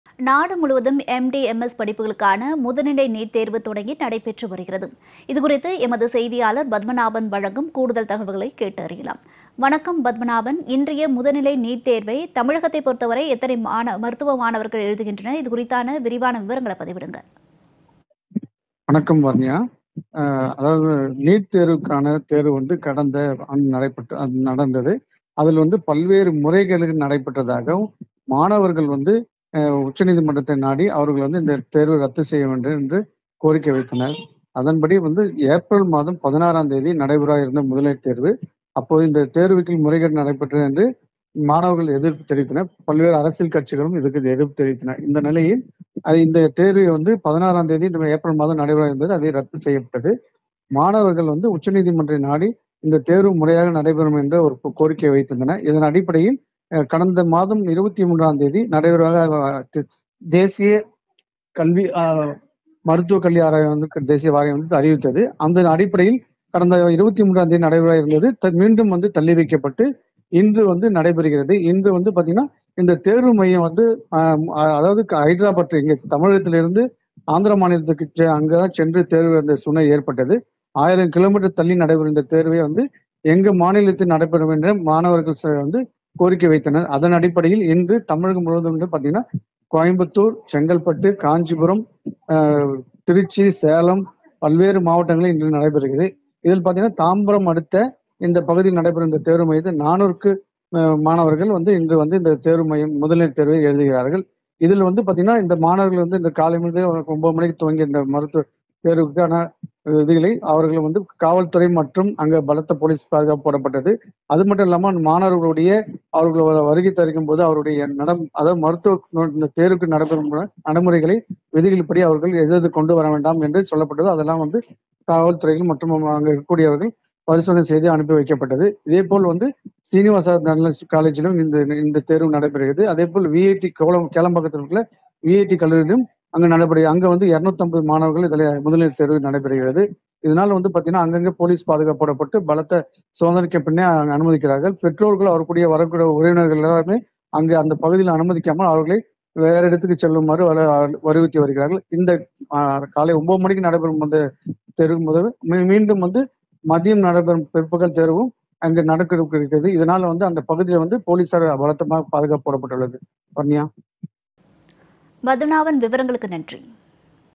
முதுநிலை மருத்துவப் படிப்புகளுக்கான நீட் தேர்வு நாடு முழுவதும் விறுவிறுப்பாக நடைபெற்று வருகிறது
முதுநிலை மருத்துவப் படிப்புகளுக்கான நீட் தேர்வு நாடு முழுவதும் விறுவிறுப்பாக நடைபெற்று வருகிறது | MDMS | Neet | Jaya plus